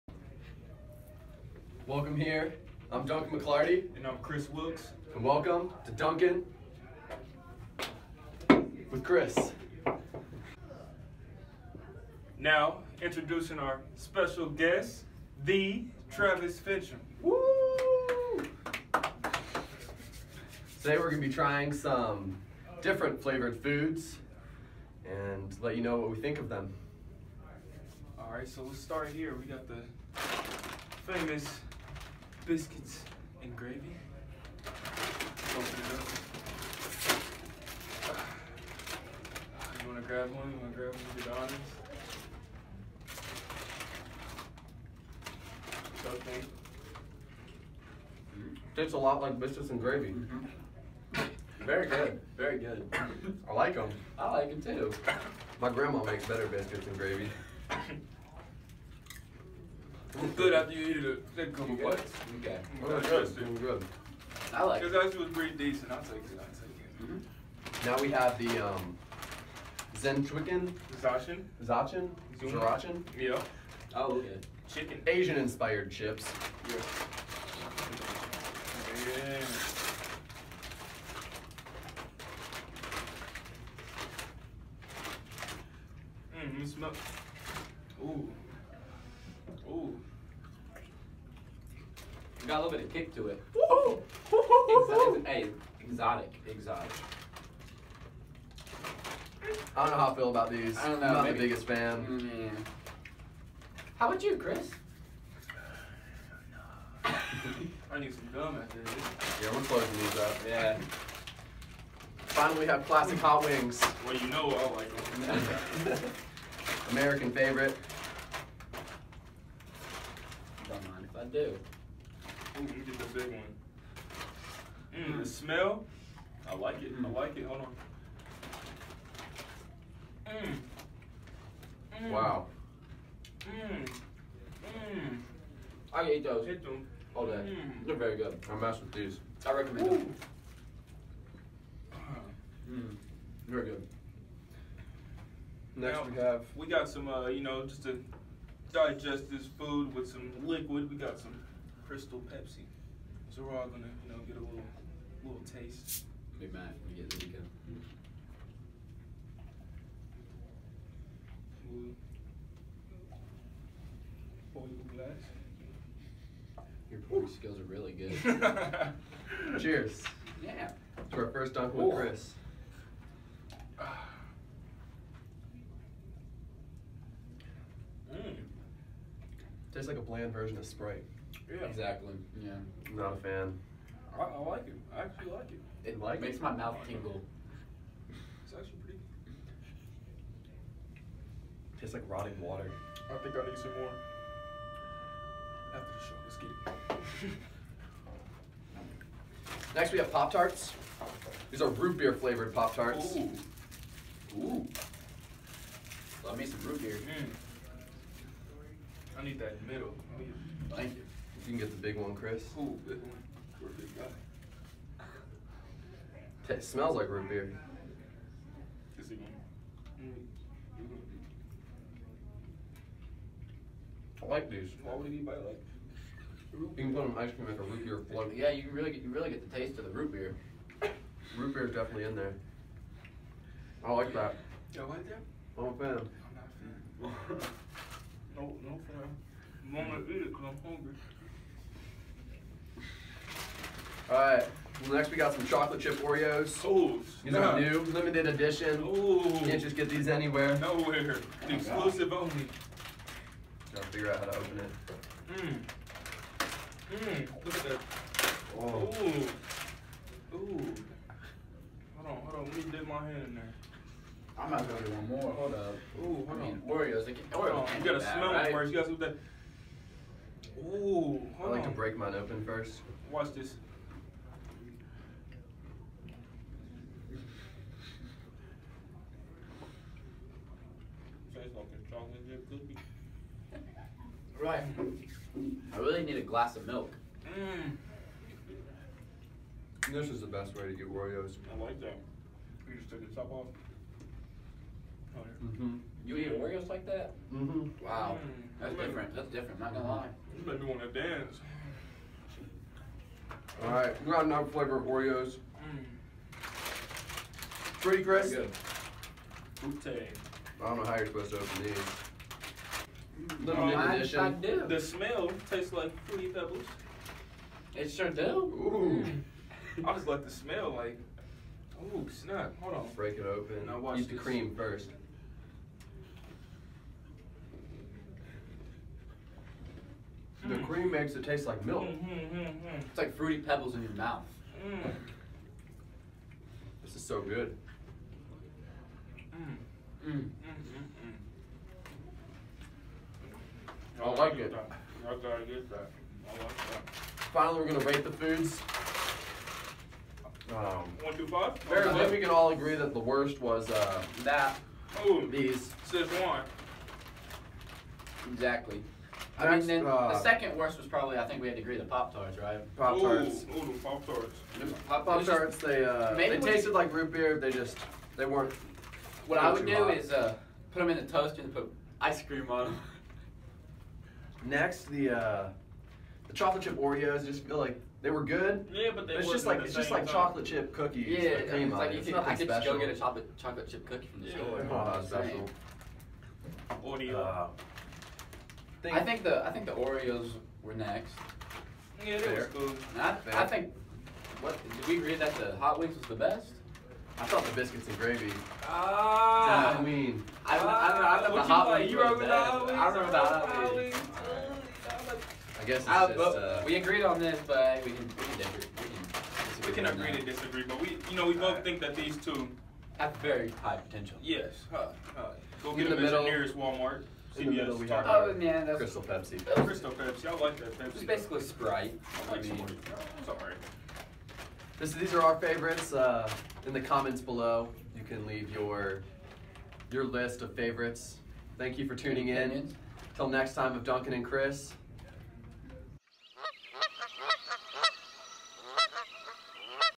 Duncan with Kris: Fun Foods
Duncan McLarty and Kris Wilkes star in "Duncan with Kris!" This first episode, Duncan and Kris are joined by Travis Finchum as they try a variety of new food products!